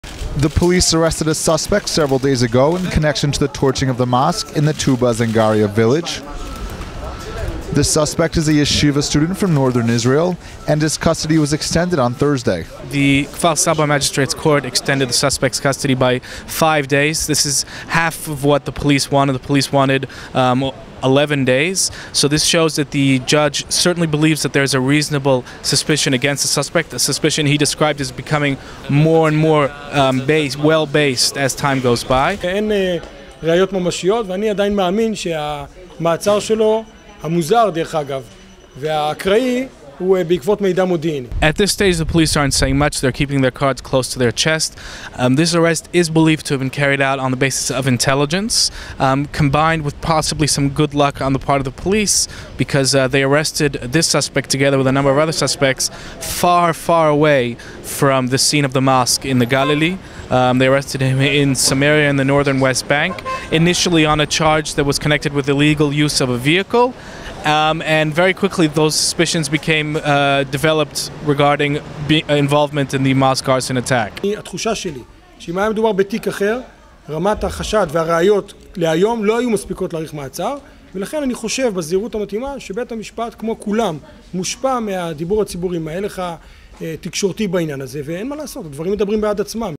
Gag order on mosque torching suspect partially lifted
"Price tag" suspect is yeshiva student from northern Israel; police say forensic evidence found at scene of crime.